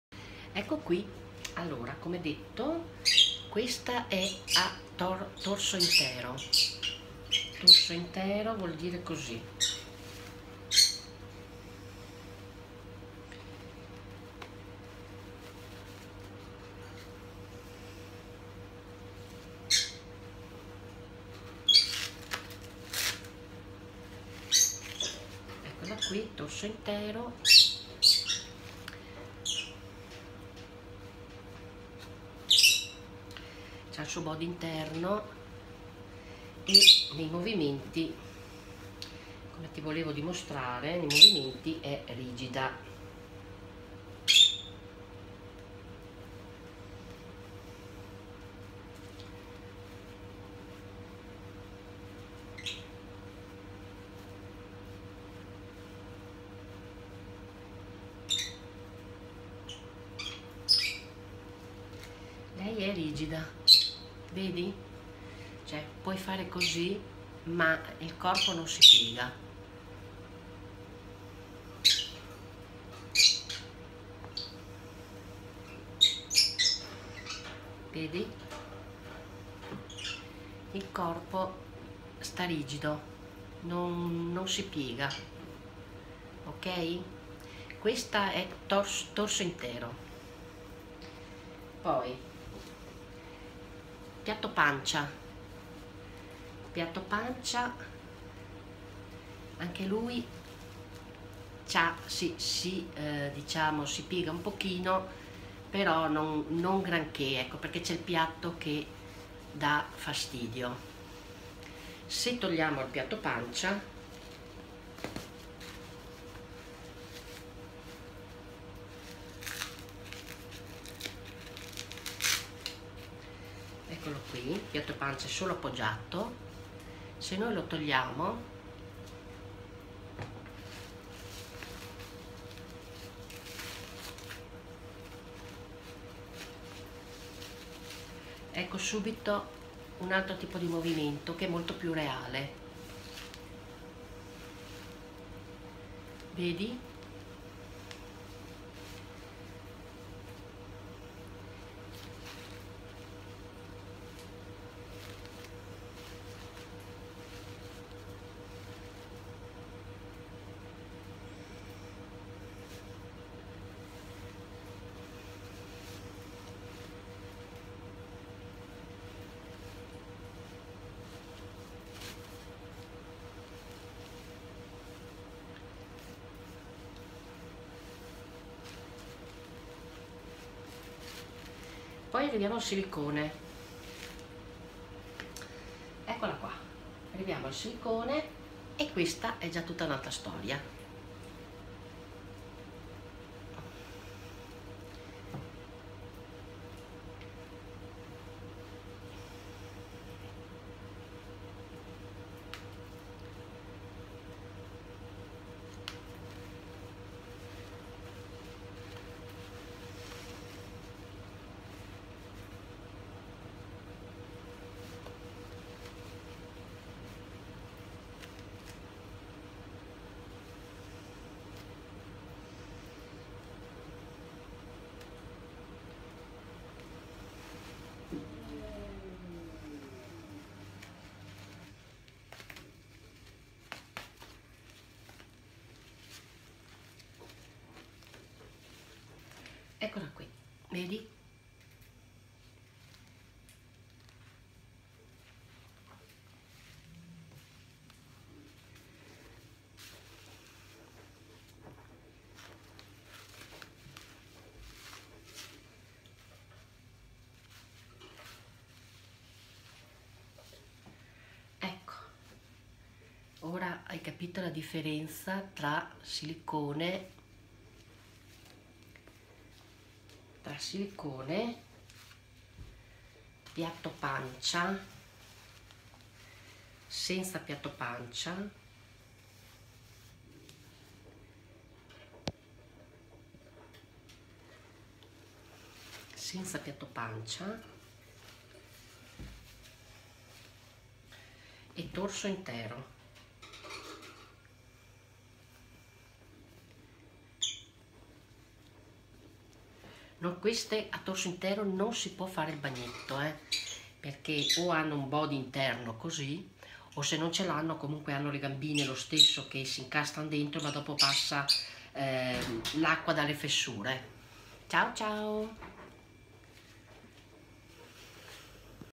Reborn e silicone movimenti
Differenze di movimentazione tra reborn full body , con piatto pancia e senza piatto è un silicone, buona visione